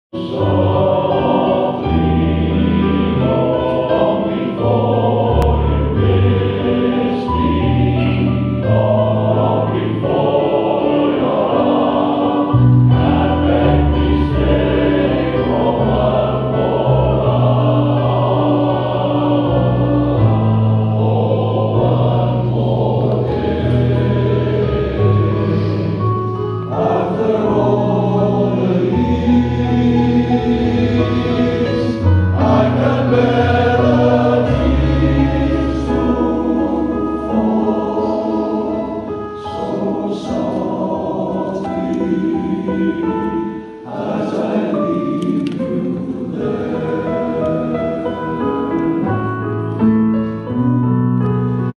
Practice for the annual concert.
We're busy practicing for our first annual concert in 3 years. Visit our Facebook page or Website for details